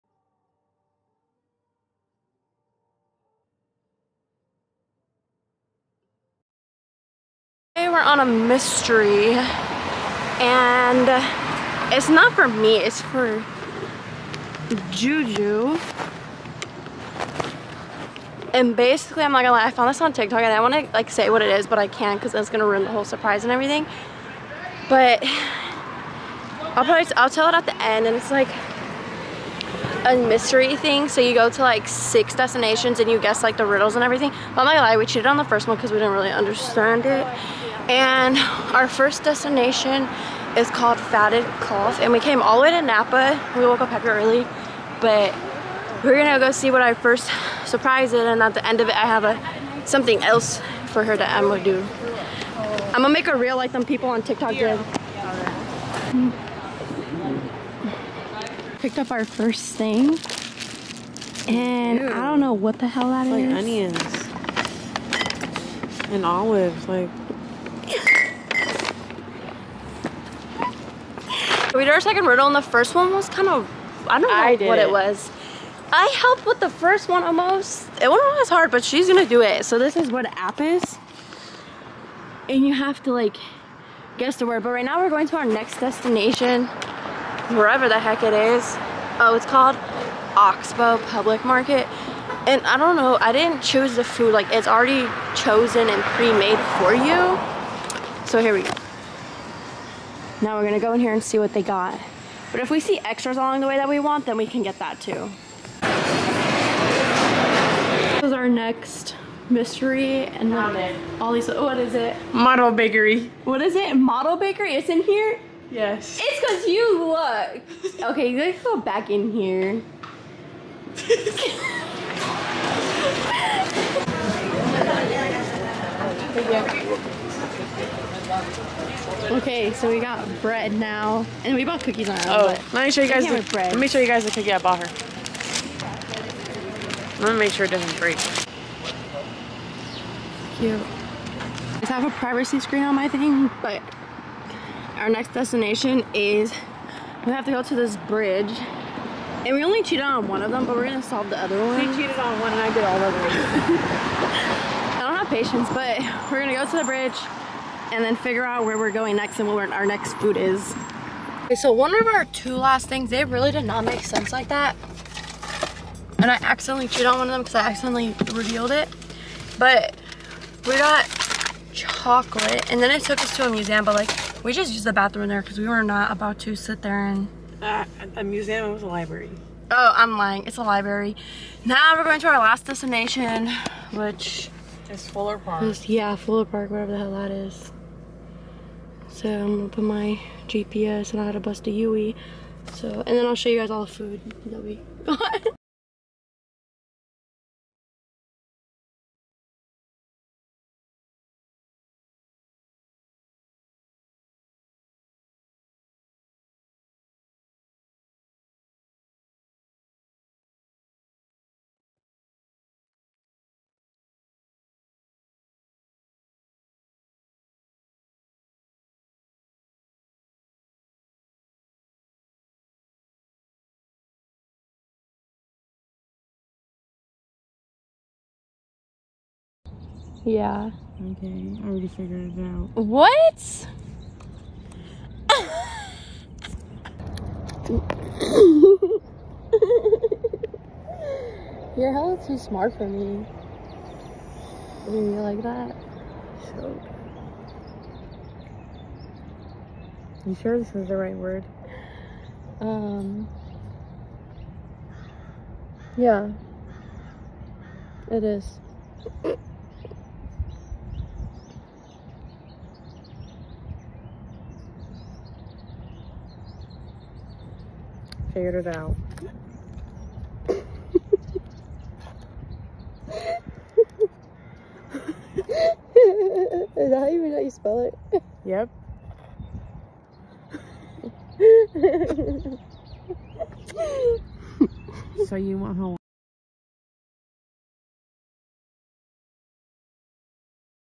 Surprising her for Valentine’s Day 😍
my babyyy🥰